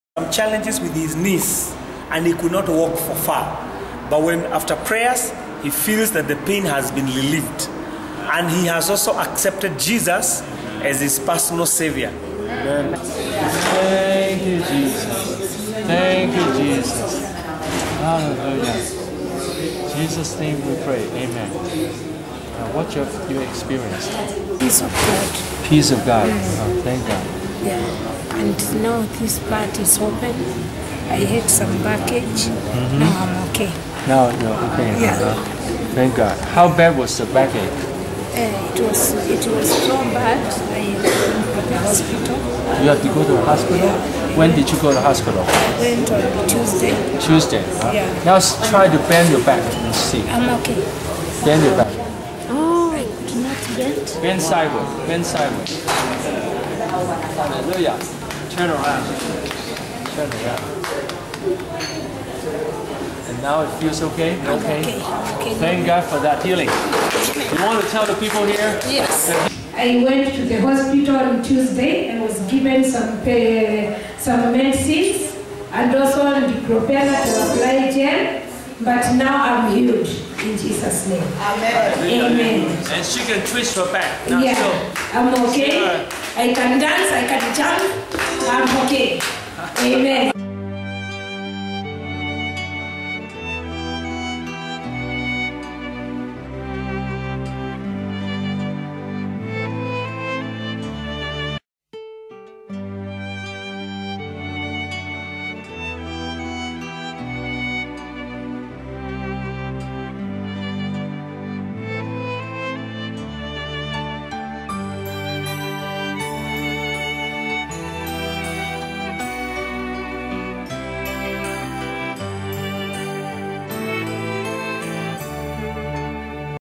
Sharing about experiencing healing during prayer-Kenya-in English-Pastor Yip
Sharing about experiencing healing-Kenya-in English
I am willing to help you or your church to experience spiritual revival and to do training for ministry for free.